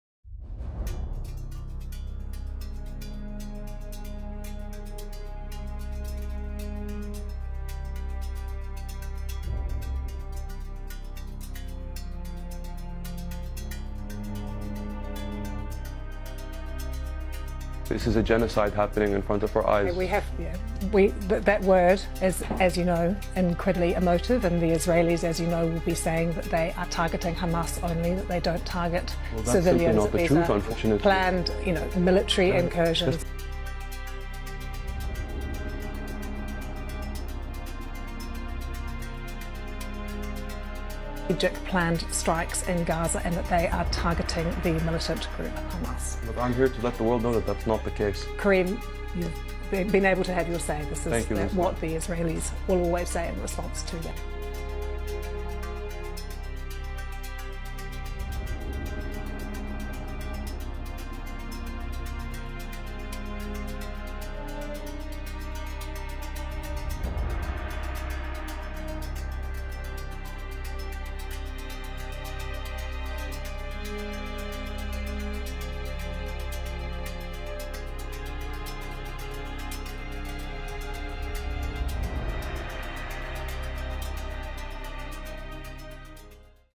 BBC Presenter Cuts Off Palestinian Guest Describing Gaza
BBC presenter Lucy Hockings sparked outrage after she repeatedly interrupted Palestinian guest Karim Ali after he described the war on Gaza as genocide and said that Israel was bombing the enclave indiscriminately.